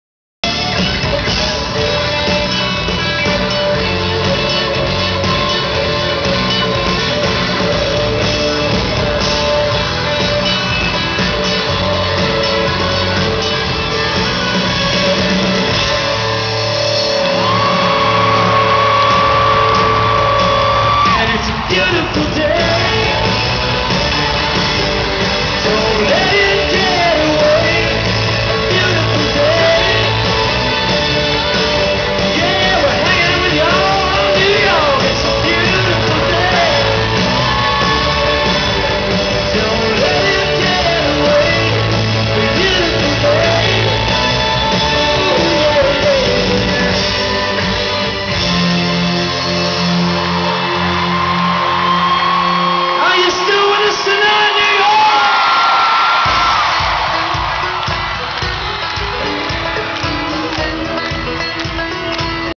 keith urban
the first keith urban concert i went to.